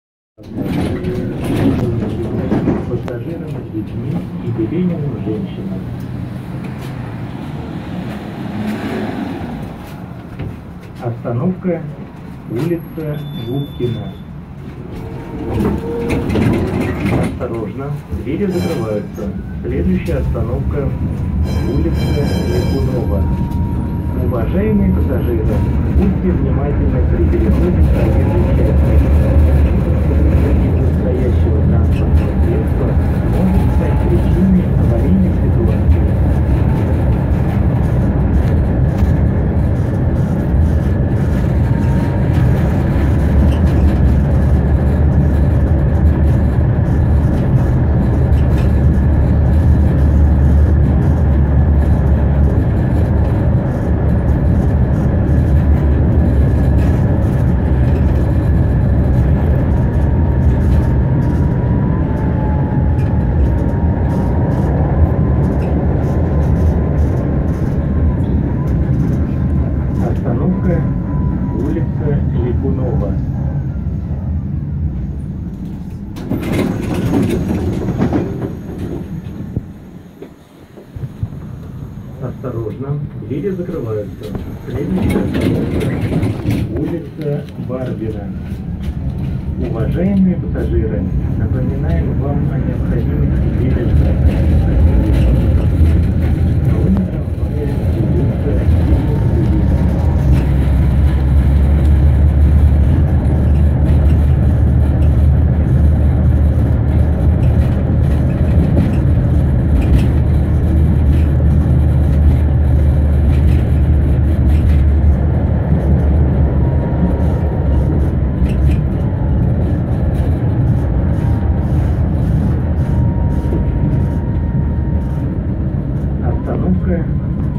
улица Вавилова ЖК "Вавилов Дом" Москва 17 октября 2018 г.
на трамвае по улице Вавилова в сторону центра